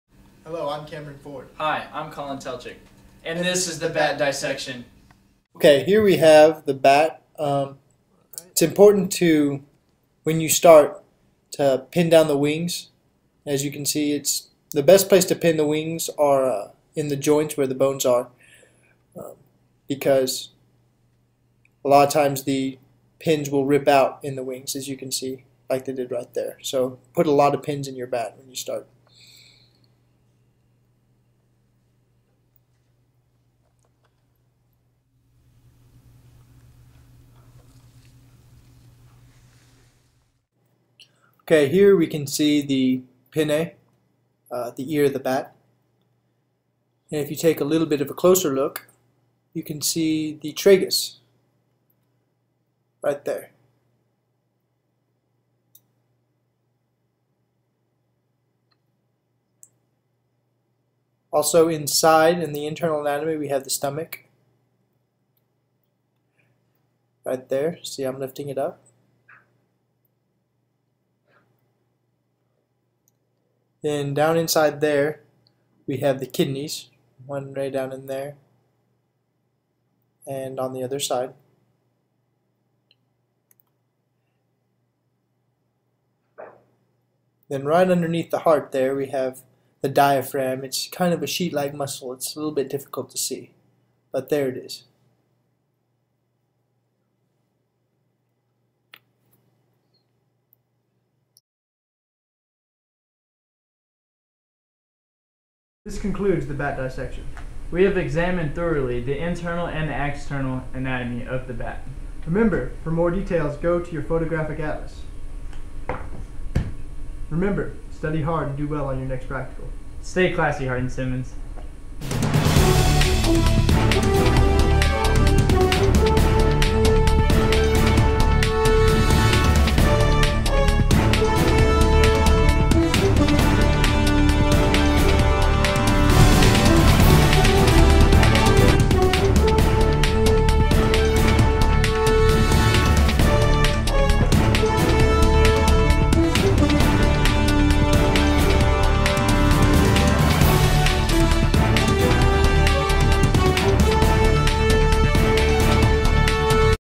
Hardin-Simmons Biology Department Bat Dissection Video
This is the official Hardin-Simmons Bat Dissection video. We examine throughly the internal and external anatomy of the bat. Remember study hard, and do well on your next lab practical. Stay Classy Hardin-Simmons.